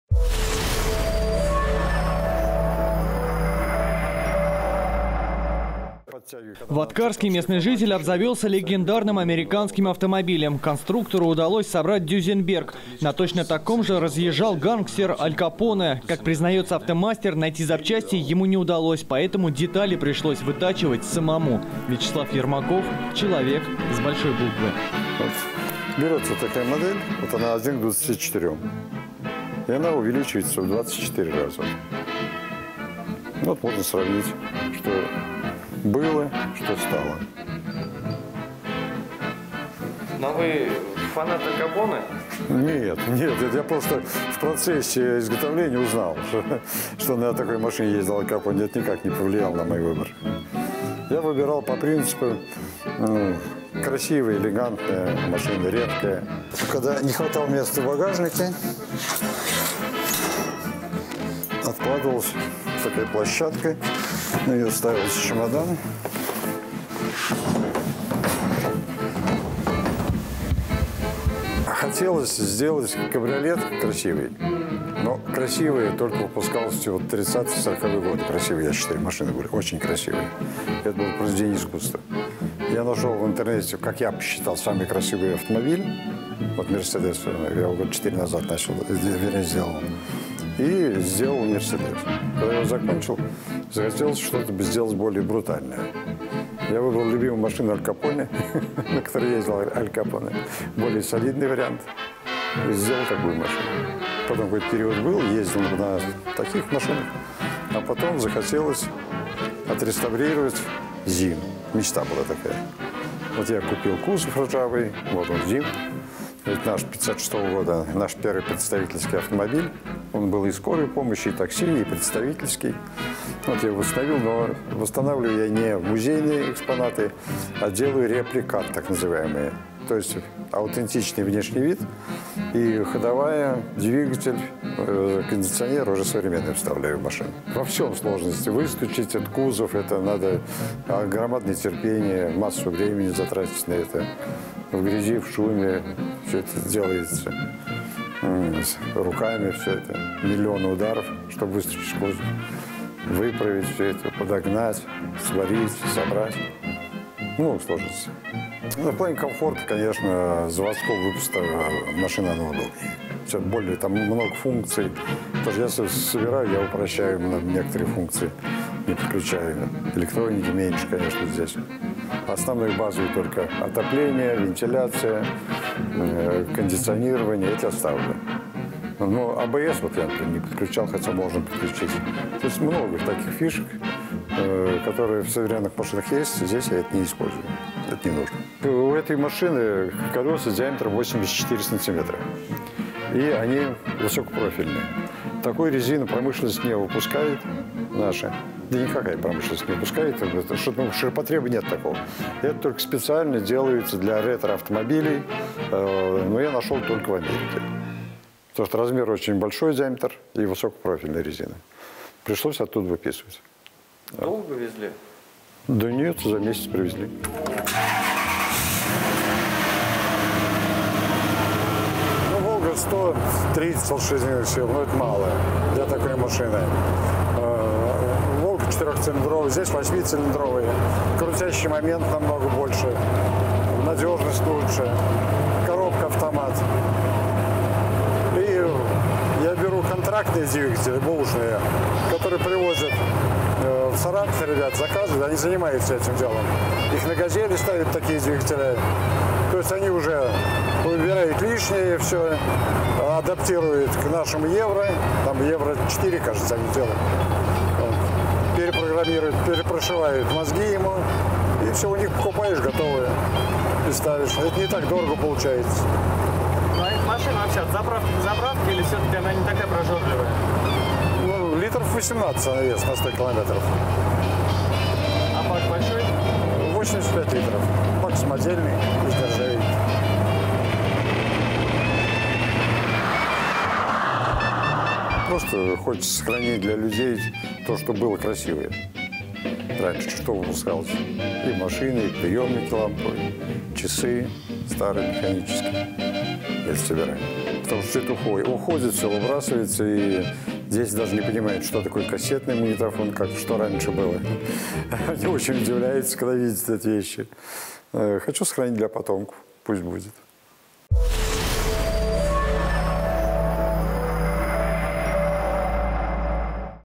Саратовец сам собирает ретроавтомобили | Легендарные автомобили
В Аткарске местный житель обзавёлся легендарным американским автомобилем. Конструктору удалось собрать "Дюзенберг". На точно таком же разъезжал гангстер Аль Капоне. Как признается автомастер, найти запчасти ему не удалось, поэтому детали пришлось вытачивать самому. Вячеслав Ермаков - человек с большой буквы.